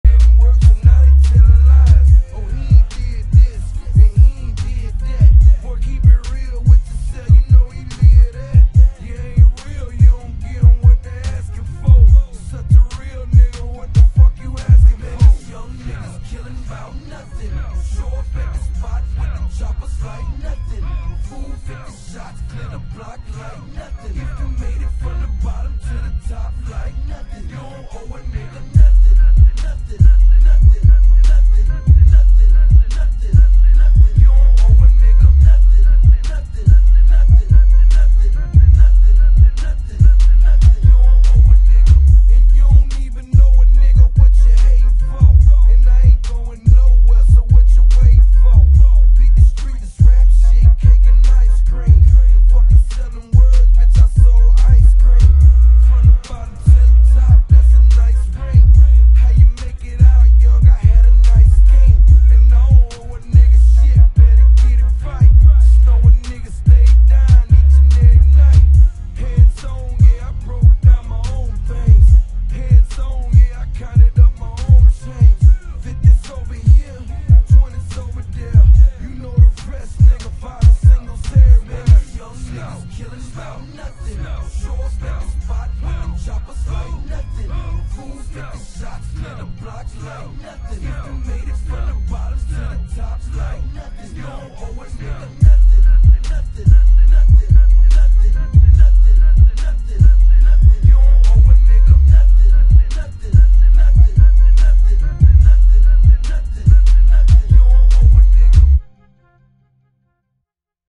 How I Bass Boost Music (Read Description)
First off when playing this video i would recommend using headphones, and turn your volume up most of the way! Now some audio enhancements will very depending on the computer you have! This was for demo purposes! Just showing an example on how to bass boost your audio!

***Social Media***